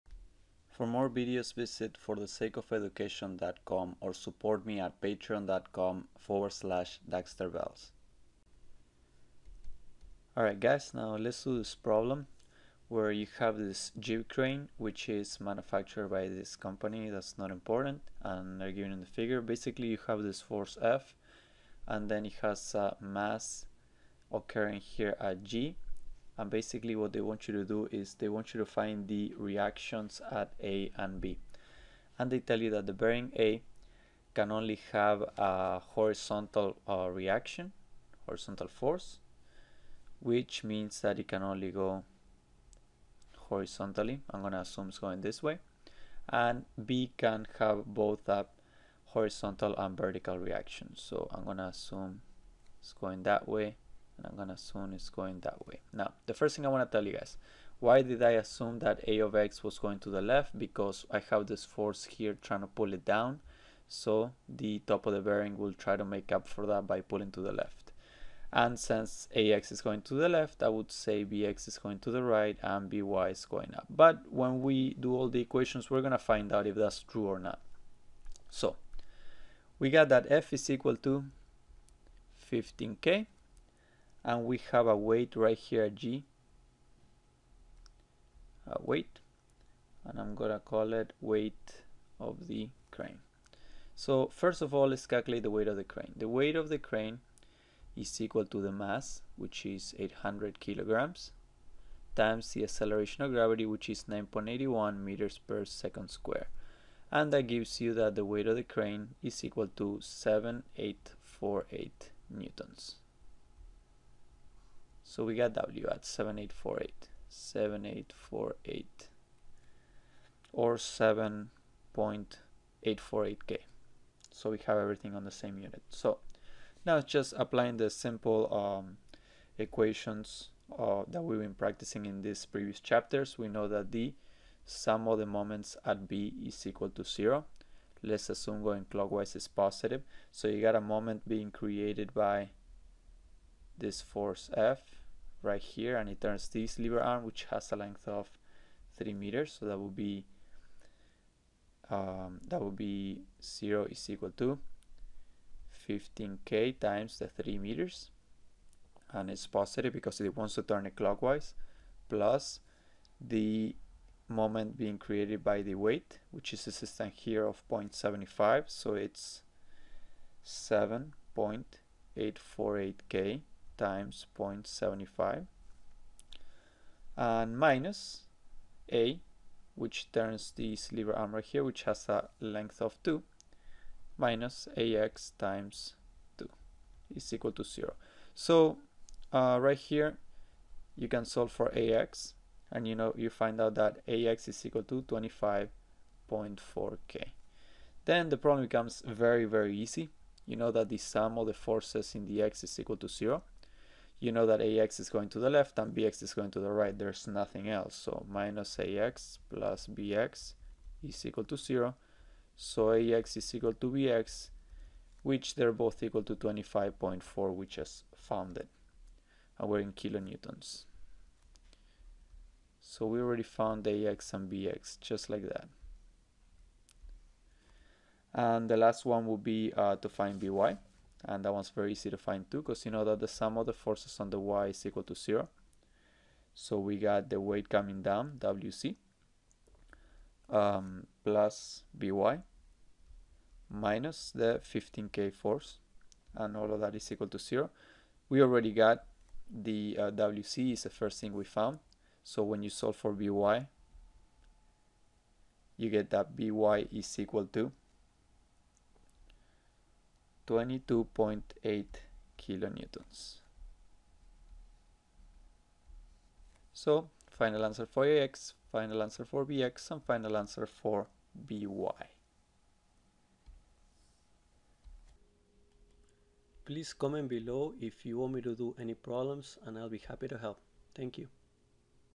Determine the reactions at its bearings
The dimensions of a jib crane, which is manufactured
by the Basick Co., are given in the figure. If the crane has a
mass of 800 kg and a center of mass at G, and the maximum
rated force at its end is F = 15 kN, determine the reactions
at its bearings. The bearing at A is a journal bearing and
supports only a horizontal force, whereas the bearing at B is
a thrust bearing that supports both horizontal and vertical
components.